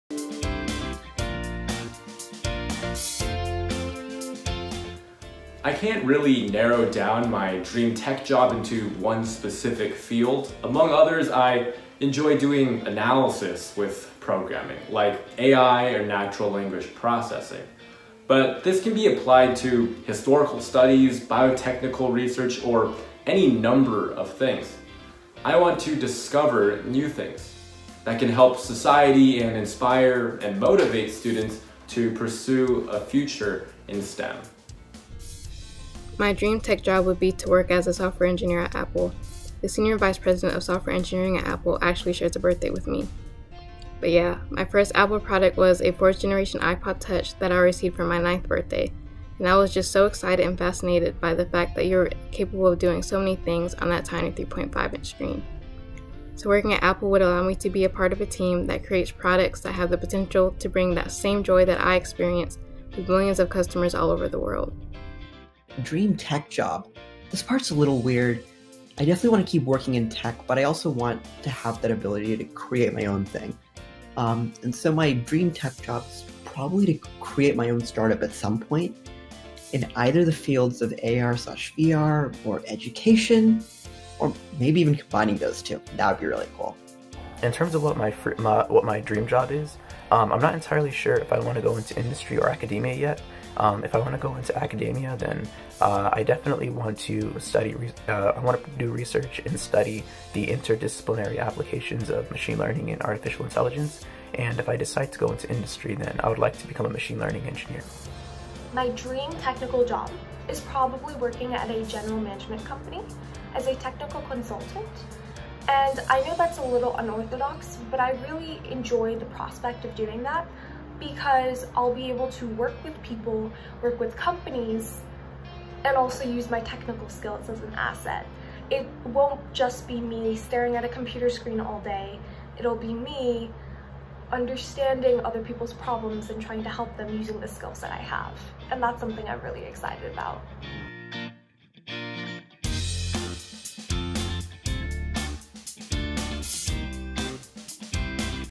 2020 Alumni Advisory Board: Dream Jobs
The 2020 Alumni Advisory Board members talk about their dream job and goals in the tech field. We love seeing such a diversity of aspirations in our group.